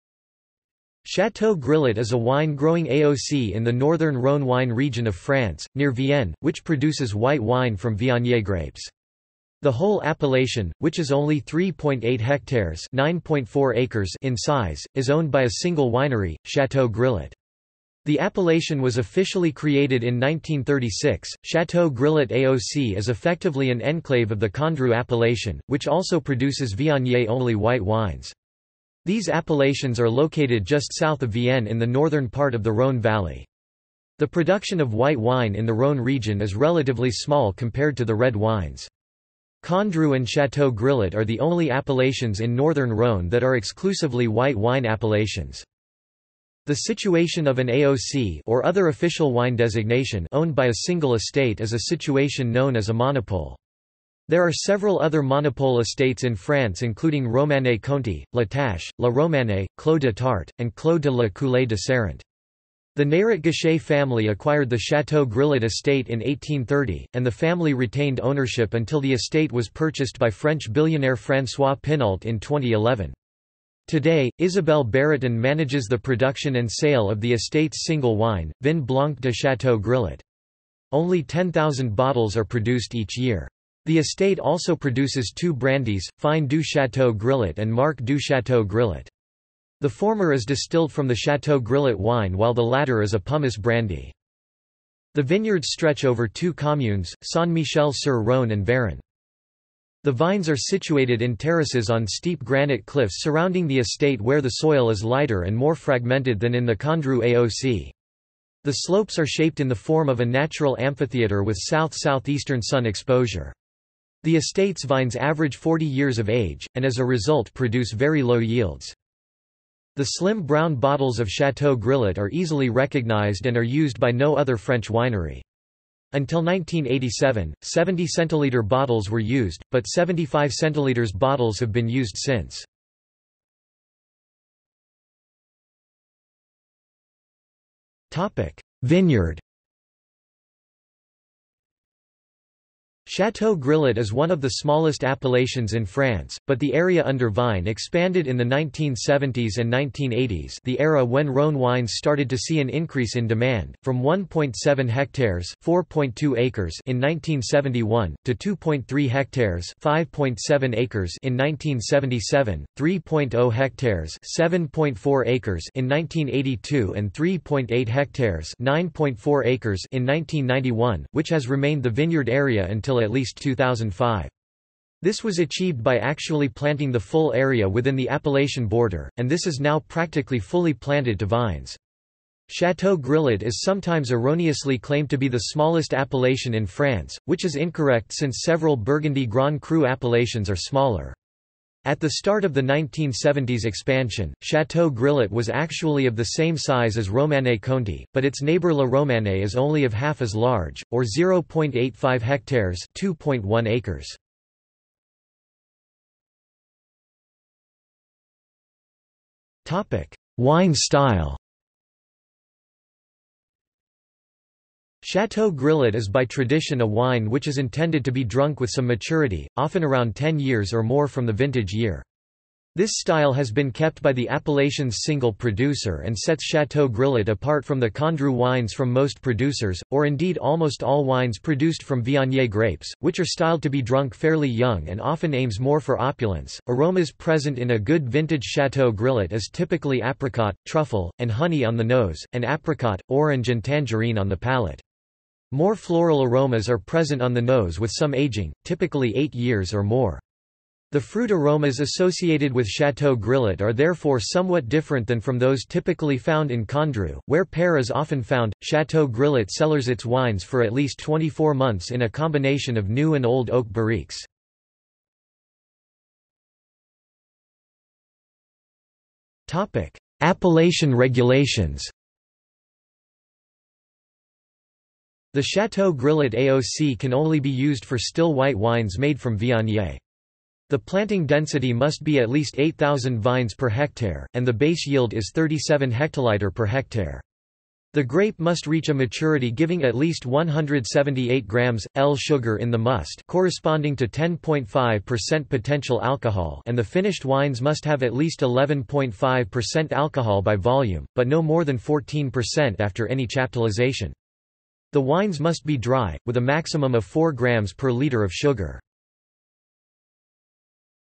Château-Grillet AOC | Wikipedia audio article
This is an audio version of the Wikipedia Article:
Château-Grillet AOC

00:02:14 1 Vineyard
00:03:26 2 Wine style
00:04:26 2.1 Appellation regulations

- Socrates

SUMMARY
Château-Grillet is a wine-growing AOC in the northern Rhône wine region of France, near Vienne, which produces white wine from Viognier grapes. The whole appellation, which is only 3.8 hectares (9.4 acres) in size, is owned by a single winery, Château-Grillet. The appellation was officially created in 1936.Château-Grillet AOC is effectively an enclave of the Condrieu appellation, which also produces Viognier-only white  wines. These appellations are located just south of Vienne in the northern part of the Rhône valley. The production of white wine in the Rhône region is relatively small compared to the red wines. Condrieu and Château-Grillet are the only appellations in northern Rhône that are exclusively white wine appellations.
The situation of an AOC (or other official wine designation) owned by a single estate is a situation known as a monopole. There are several other monopole estates in France including Romanée-Conti, La Tâche, La Romanée, Clos de Tart, and Clos de la Coulée de Serrant. The Neyret-Gachet family acquired the Château-Grillet estate in 1830, and the family retained ownership until the estate was purchased by French billionaire François Pinault in 2011. Today, Isabelle Baratin manages the production and sale of the estate's single wine: Vin Blanc de Château-Grillet. Only 10,000 bottles are produced each year. The estate also produces two Brandies: Fine du Château-Grillet and Marc du Château-Grillet. The former is distilled from the Château-Grillet wine while the latter is a pomace brandy.
The vineyards stretch over two communes: Saint-Michel-sur-Rhône and Vérin.
The vines are situated in terraces on steep granite cliffs surrounding the estate where the soil is lighter and more fragmented than in the Condrieu AOC. The slopes are shaped in the form of a natural amphitheater with south-southeastern sun exposure. The estate's vines average 40 years of age, and as a result produce very low yields.
The slim brown bottles of Château-Grillet are easily recognised and are used by no other French winery. Until 1987, 70 centiliter bottles were used, but 75 cl bottles have been used since.